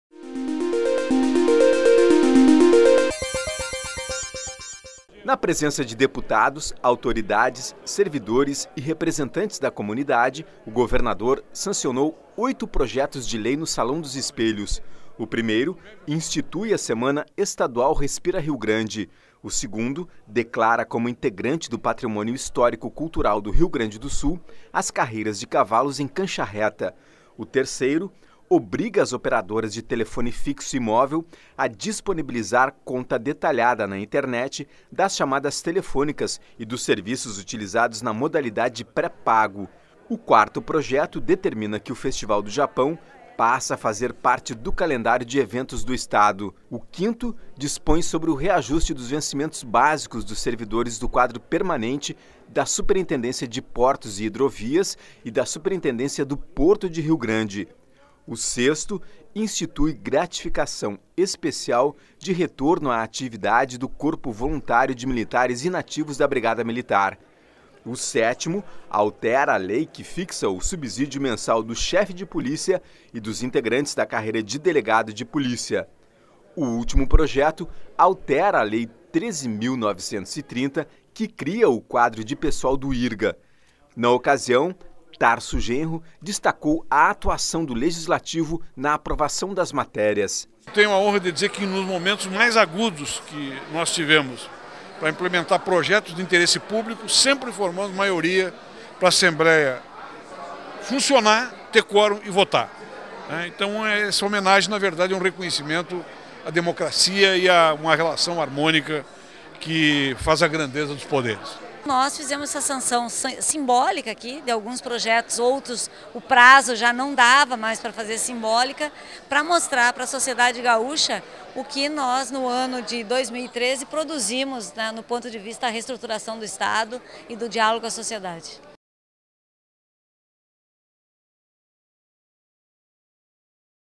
Governador Tarso Genro sanciona oito projetos de lei
Na presença de deputados, autoridades, servidores e representantes da comunidade, o governador Tarso Genro sancionou oito projetos de lei no Salão dos Espelhos nesta quarta-feira (15).